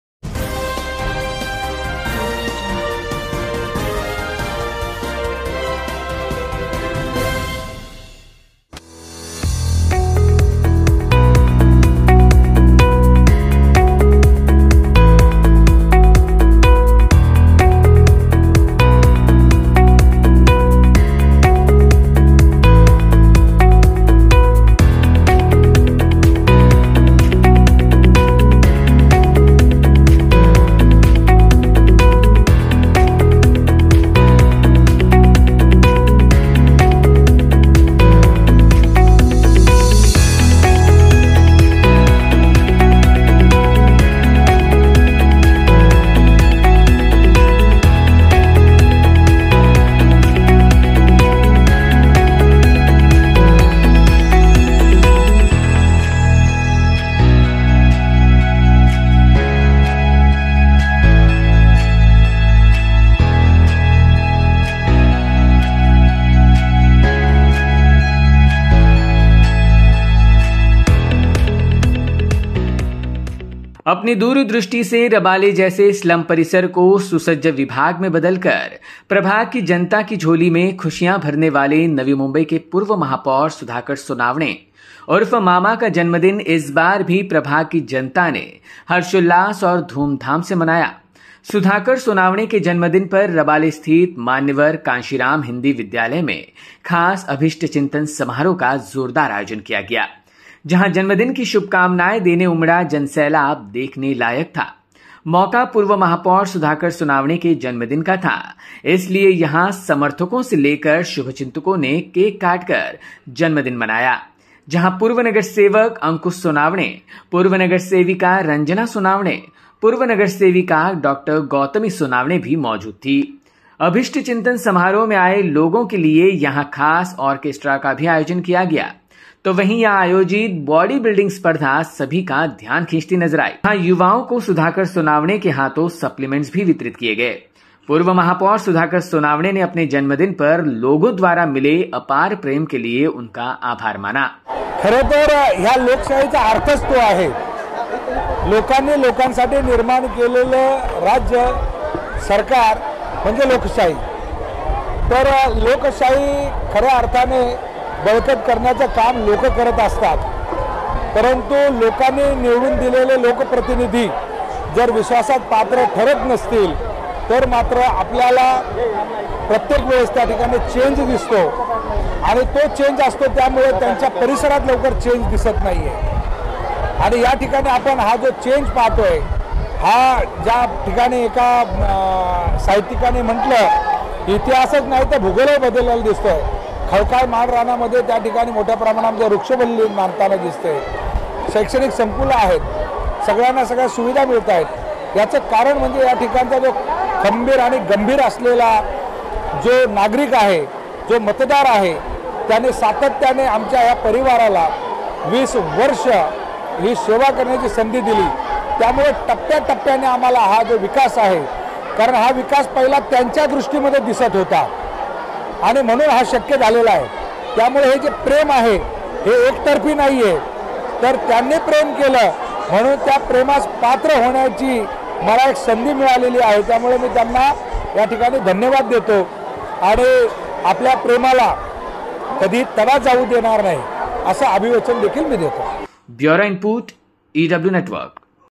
Sudhakar Sonavane Birthday | अभीष्टचिंतन समारोह में उमड़ा भारी जन सैलाब
अपनी दूर दृष्टि से रबाले जैसे slam परिसर को सुसज्ज विभाग में बदलकर प्रभाग की जनता की झोली के खुशियां भरने वाले नवी मुंबई के पूर्व महापौर सुधाकर सोनावने उर्फ मामा का जन्मदिन इस बार भी प्रभाग की जनता ने हर्षोल्लास और धूमधाम से मनाया ।सुधाकर सोनावने के जन्मदिन पर रबाले स्थित मान्यवर कांशीराम हिंदी विद्यालय में खास अभीष्ट चिंतन समारोह का जोरदार आयोजन किया गया।जहा जन्मदिन की शुभ कामनाएं देने उमड़ा जनसैलाब देखने लायक था।मौका पूर्व महापौर सुधाकर सोनावने के जन्मदिन का था इसलिए यह समर्थको से लेकर शुभ चिंतकों ने केक कटकर जन्मदिन मनाया जहा पूर्व नगरसेवक अंकुश सोनावाने,पूर्व नगरसेविका रंजना सोनावने पूर्व नगरसेविका डॉ गौतमी सोनावने भी मौजूद थी। अभिष्ठ चिंतन सोमारोह में आए लोगो के लिए यह खास ऑर्केस्ट्रा का भी आयोजन किया गया तो वही यह आयोजित बॉडी बिल्डिंग स्पर्धा सभी का ध्यान खींचती नजर आई।कहा युवाओं को सुधाकर सोनावमे के हाथो suppliments भी वितरित किए गए